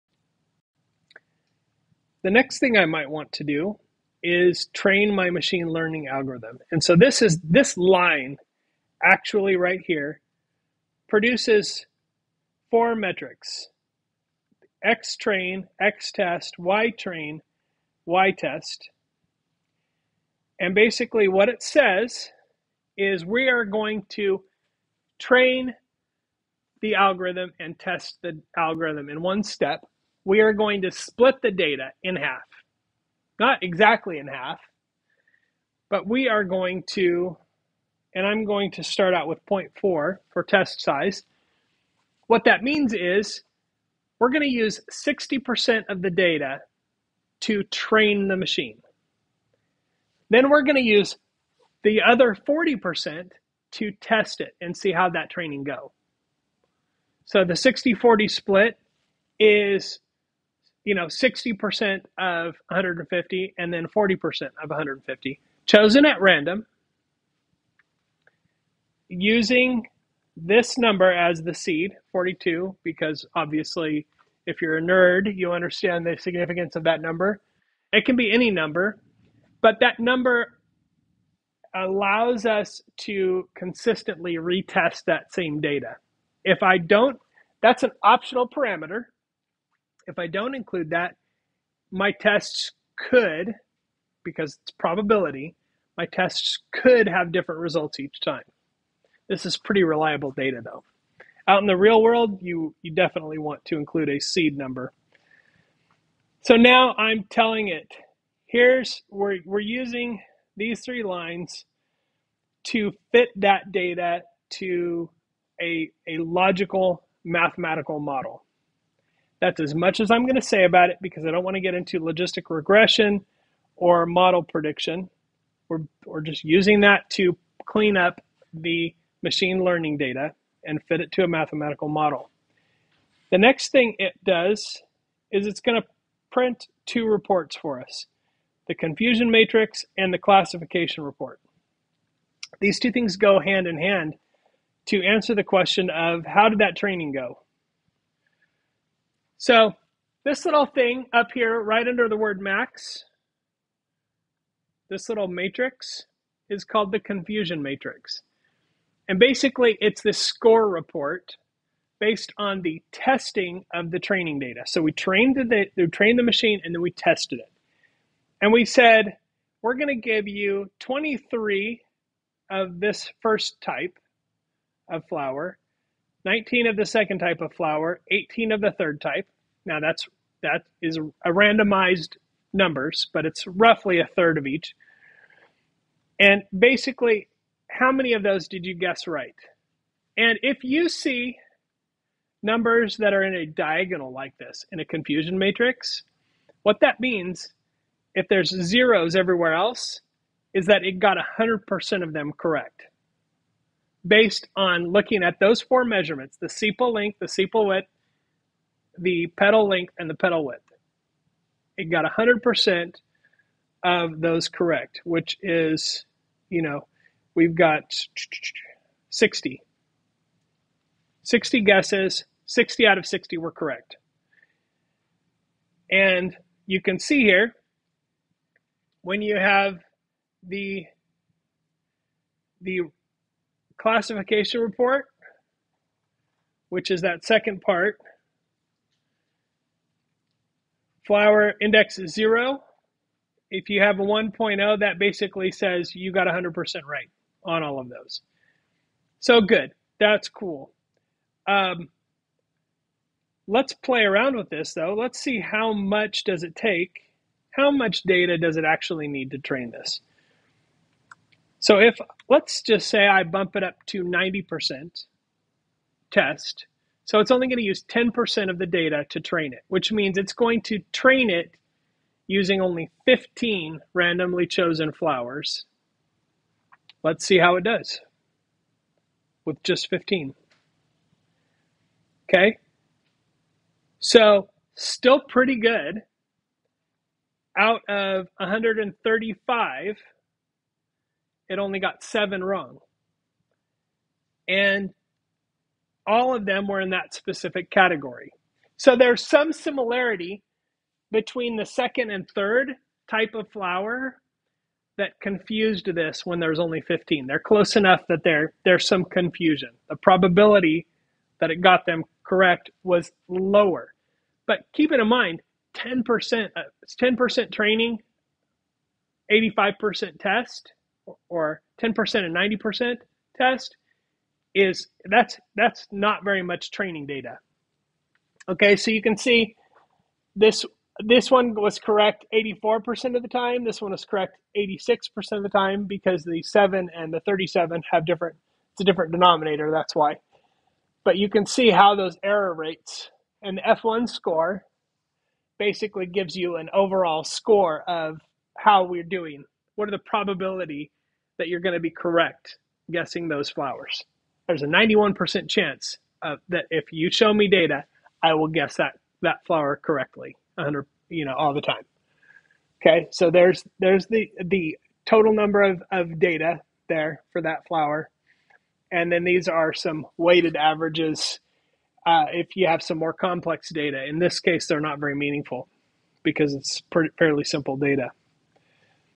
Python - Module 19 - Machine Learning, Part 2 (Spring 2024)
Data Science in Python
Python for Beginners, python for students, free python course.
This video is part of a free and open set of complete course materials
for Fundamentals of Programming Using Python

Bakersfield College - COMP B10 Course
Captured in Spring 2024, BC SouthWest Campus, BC Panorama Campus.
Programming Methods with Python